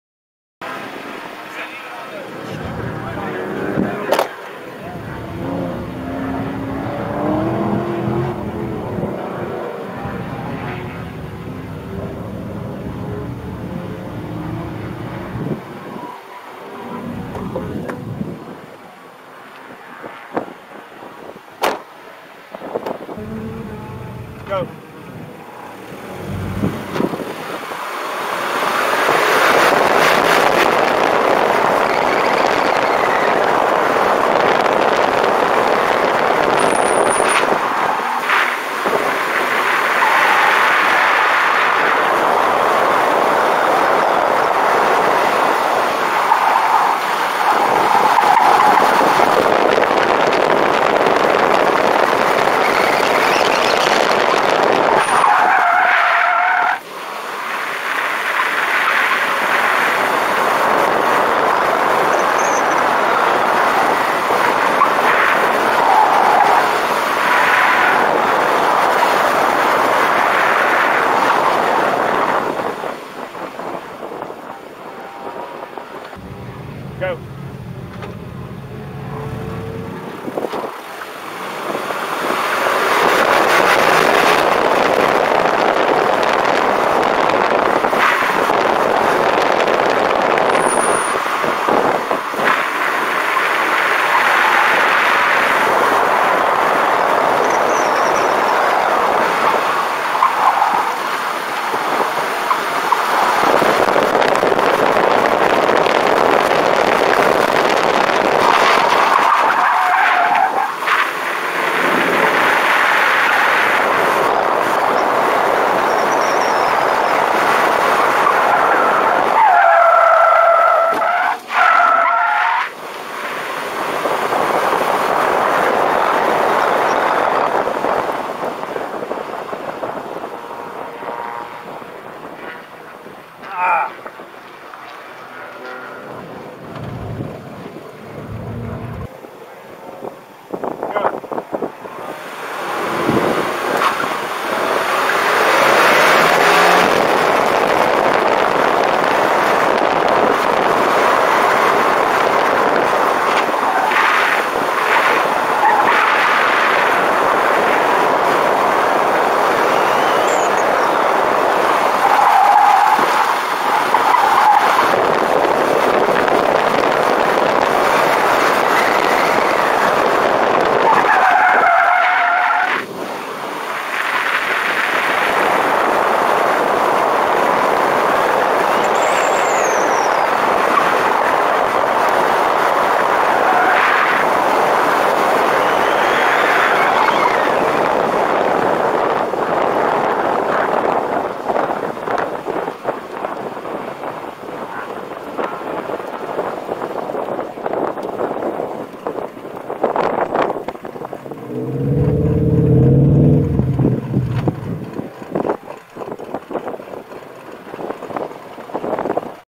Pitt Race Low Key Autocross 10/7/17 Melexis MLX90621 thermal sensor tire temperature test
Attempting to measure tire temperature with a MLX90621 16x4 thermal sensor.

Should have tried mounting the sensor on the strut body instead of the frame of the car since it moves all over the place. Also I had the 60 degree FOV sensor and the 120 degree one would have allowed me to mount it closer.